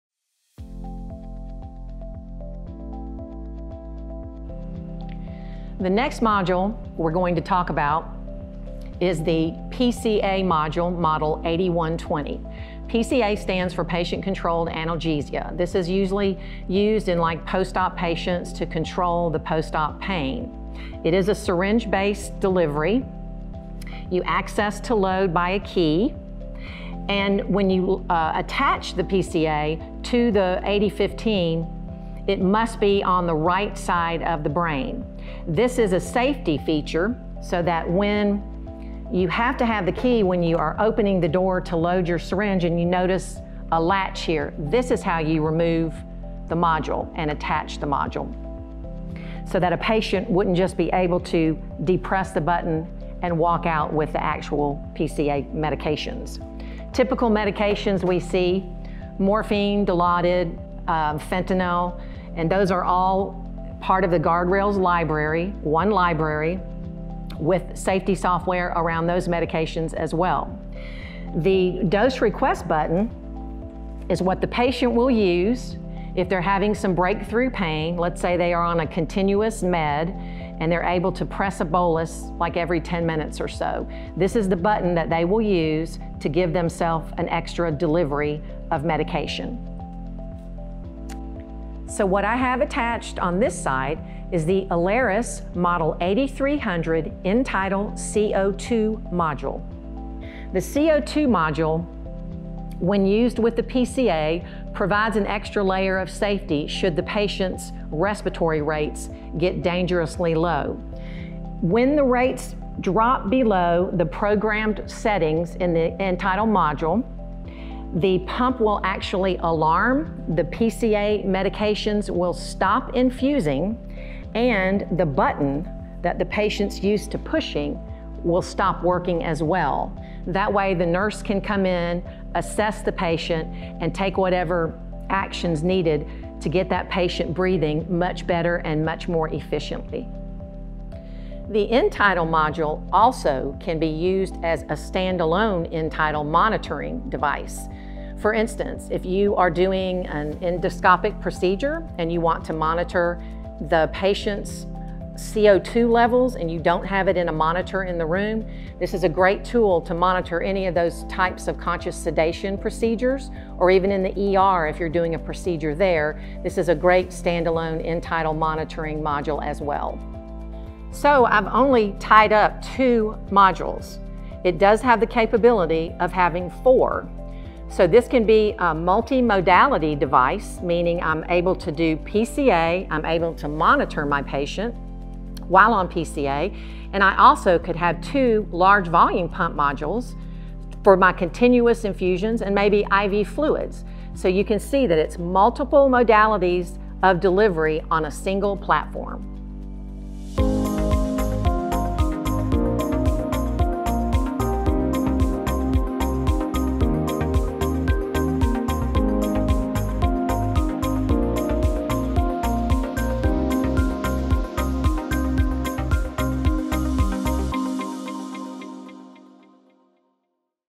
Alaris 8120 - An Introduction to the BD Alaris Infusion System
The BD Alaris 8120 is a PCA or patient-controlled analgesia device designed to provide effective and personalized pain management to patients. Patients are able to self-dose medication within a pre-set range, promoting comfort and reducing the need for frequent caregiver intervention. Combined with the ETC02 respiratory monitoring module, the respiratory rate of the patient can be monitored and helps reduce the risks of opioid-induced depression.